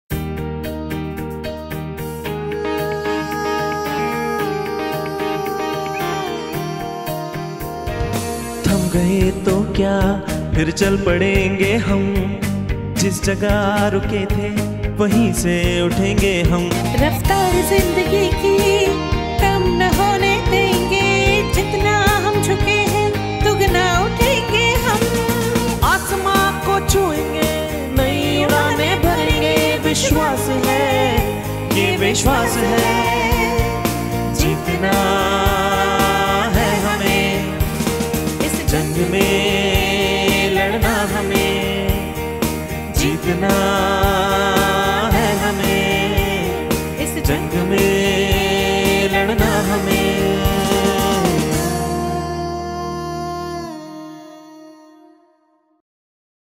Motivational Jingle By TSVS Teachers To Fight Against Corona
The Activity Department of The Sanskaar Valley School conveys a beautiful and positive message through this video during the time of the pandemic.
Through this, they are expressing the importance of social distancing and giving us hope that we shall overcome this soon.